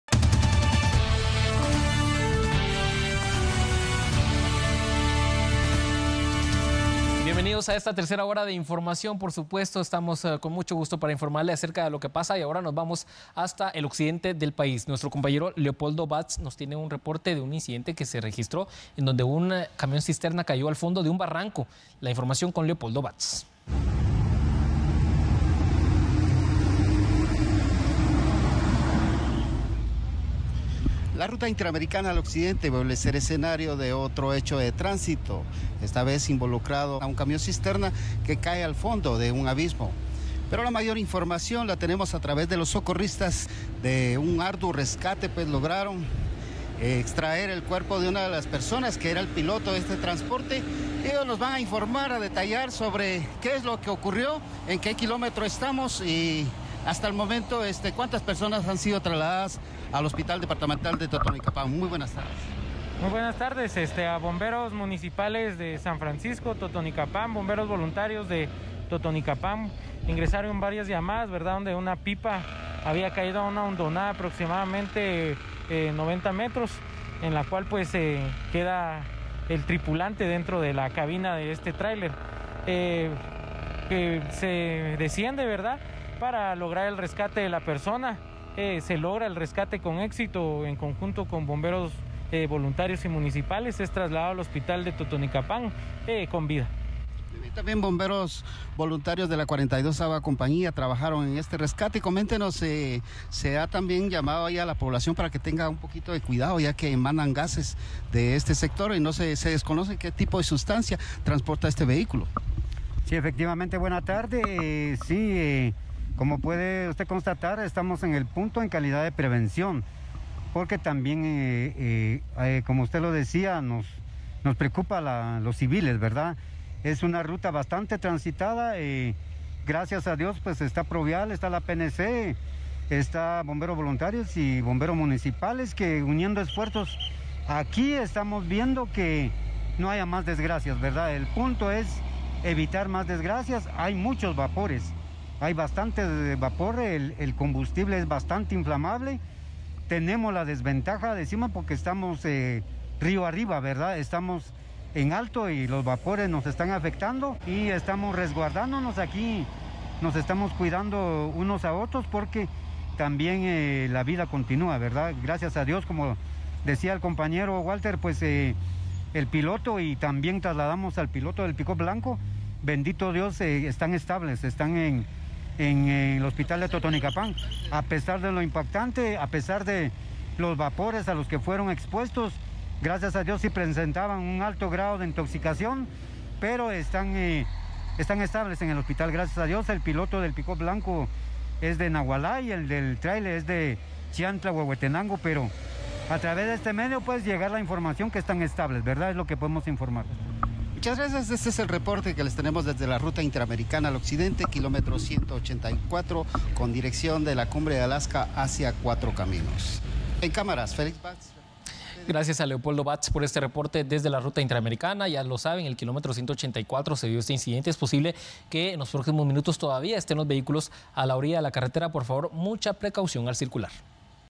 Accidente de tránsito en la ruta Interamericana dejó dos heridos
Dos personas resultaron heridas luego que un camión cisterna cayera a un abismo a la altura del kilómetro184 en la ruta Interamericana, a inmediaciones de la comunidad de Paxtoca. Las víctimas fueron trasladadas por socorristas al Hospital de Totonicapán.